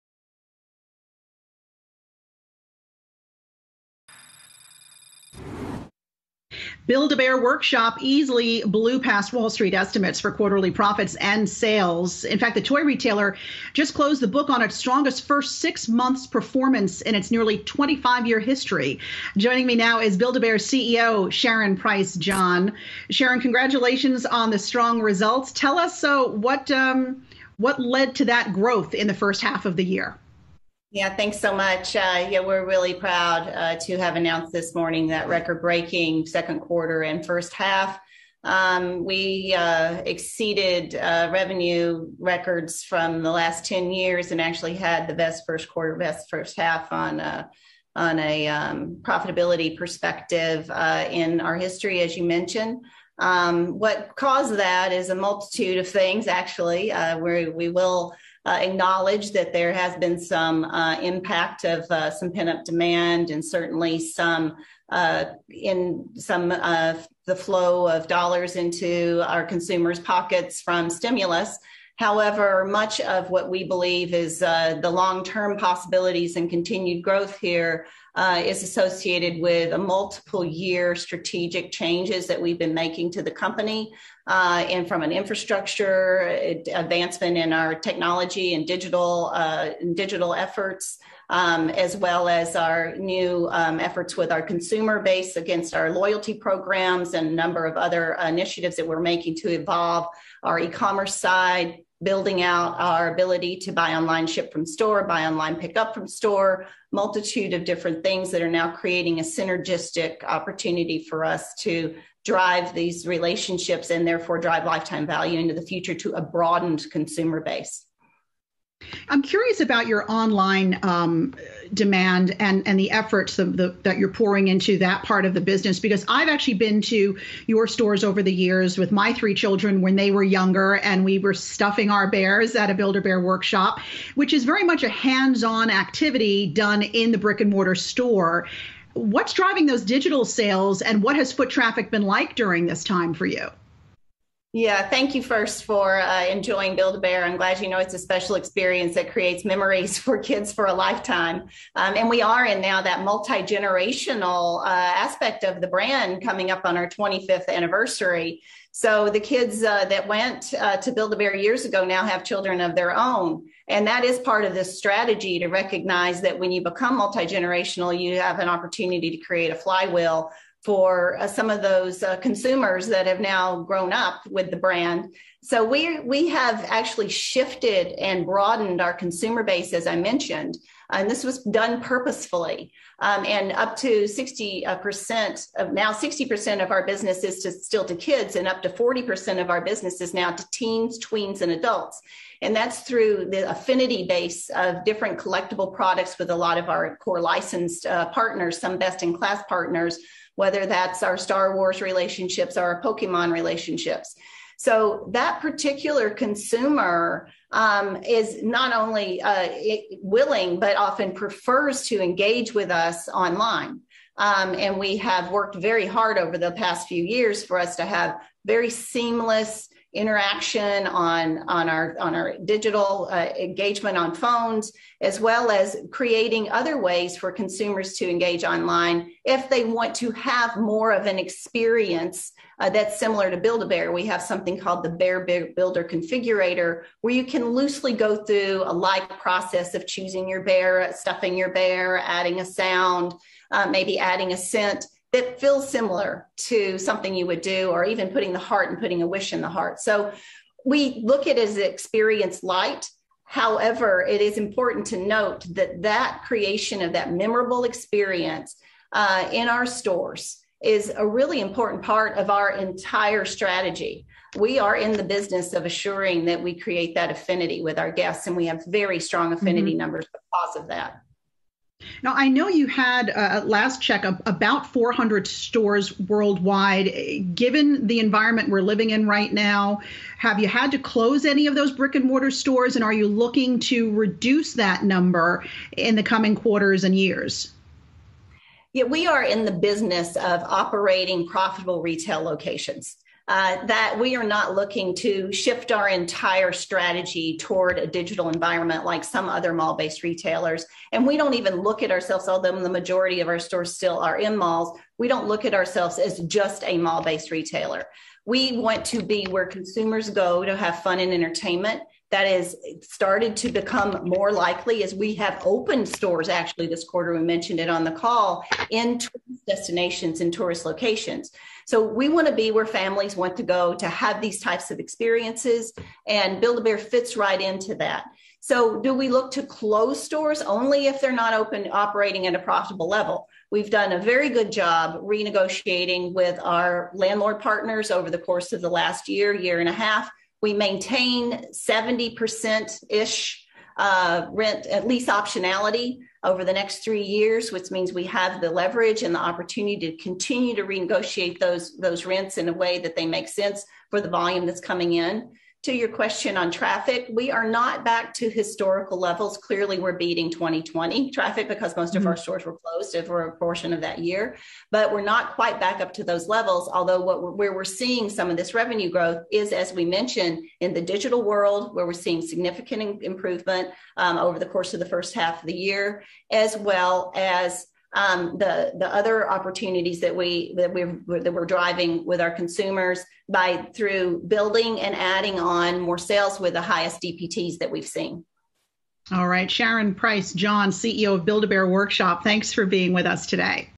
Build-a-Bear Workshop CEO on the company’s earnings beat and strongest first six months performance
Sharon Price John, CEO Build-A-Bear, joins Yahoo Finance to discuss the latest on the company’s upbeat Q2 2021 earnings and growth outlook.